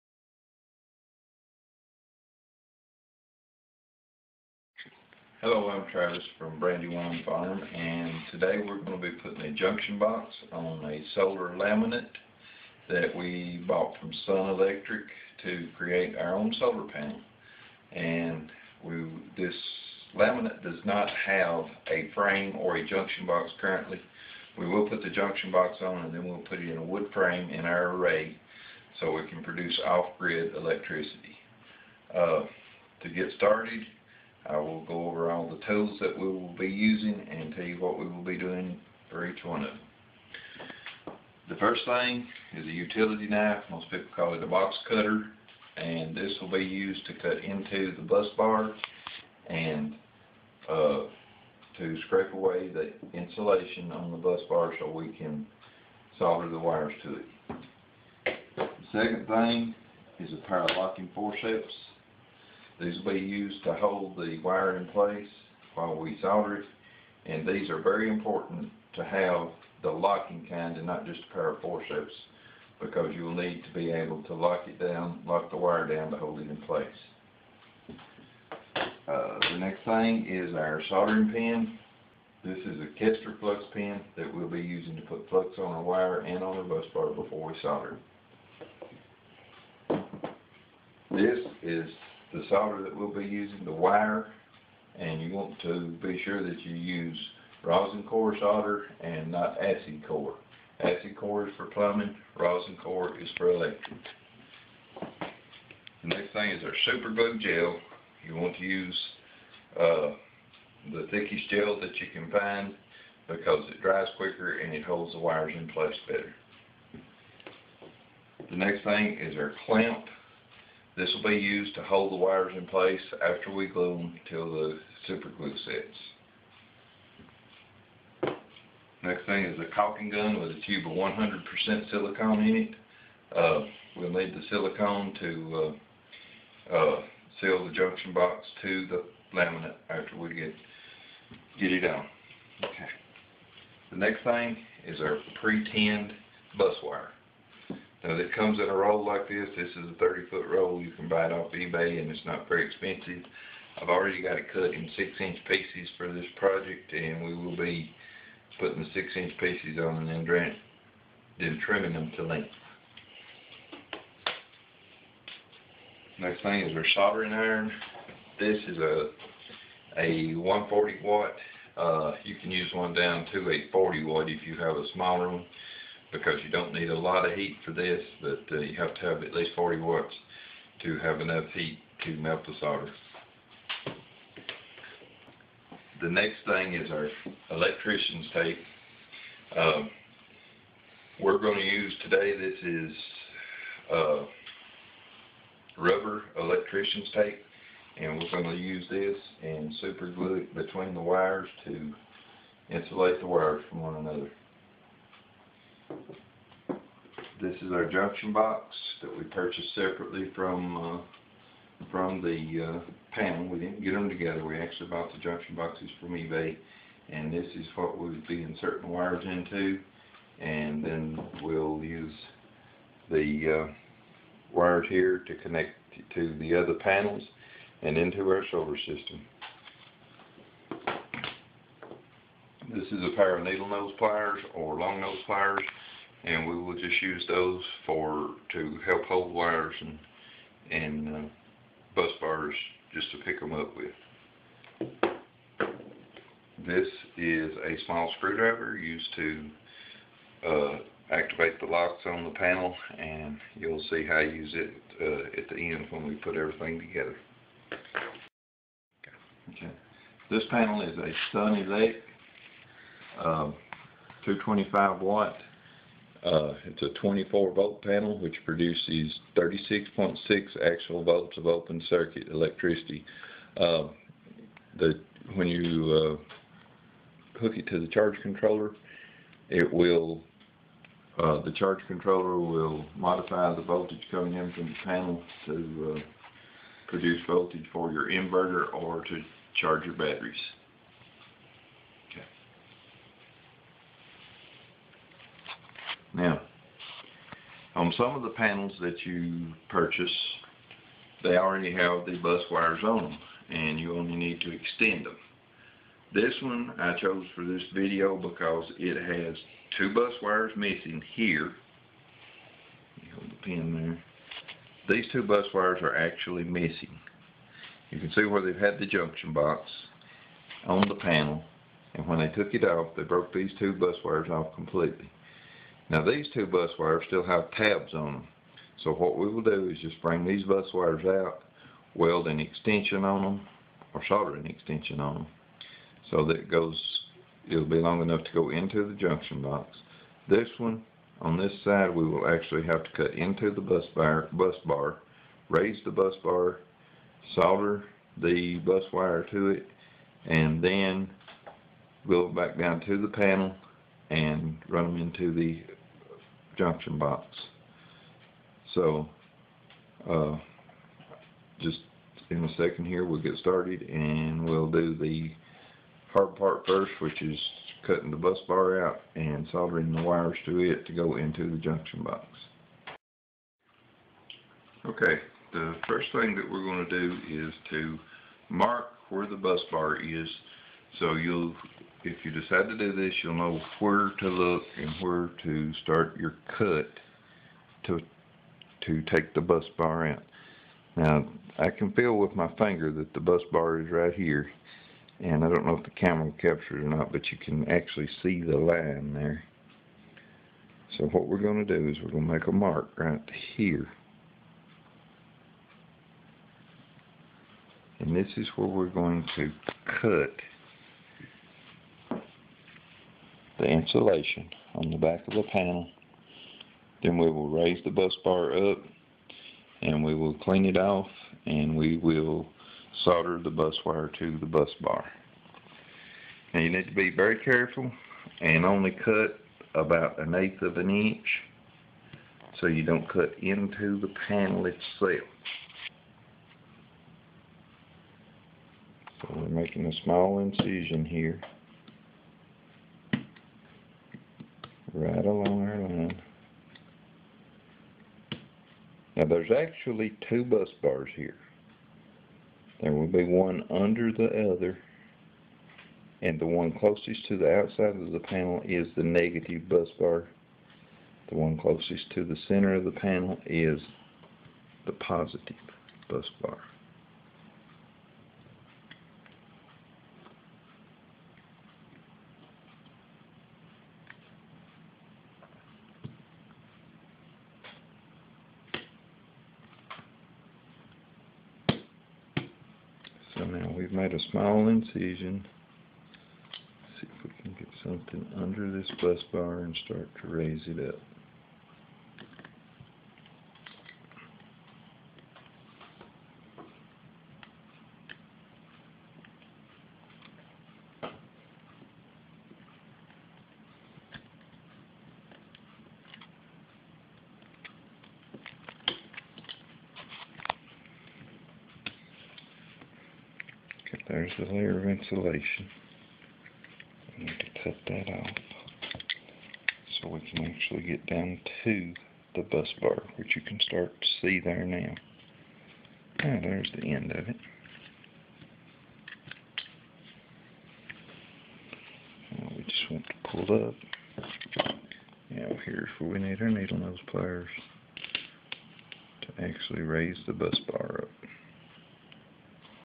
How to Install a Junction Box on a Solar Laminate I (1 of 5)
How to Install a Junction Box on a Solar Laminate. Solar Laminates are cheaper than panels because they don't have the junction boxes or a frame. They may have cosmetic damage only so they cannot be sold as a panel. This video describes how to install the junction box.